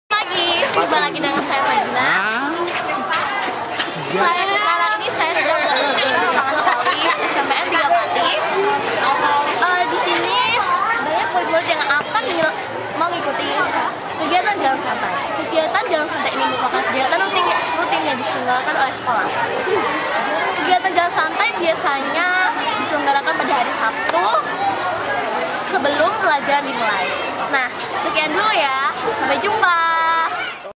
Presenter Handal Siswi SMPN 3 Pati
Pembelajaran Berbicara Bahasa Indonesia  SMP Negeri 3 Pati Kelas VIII H oleh Ananda Dona dengan guru pengampu Nyamat, S.Pd., M.Pd.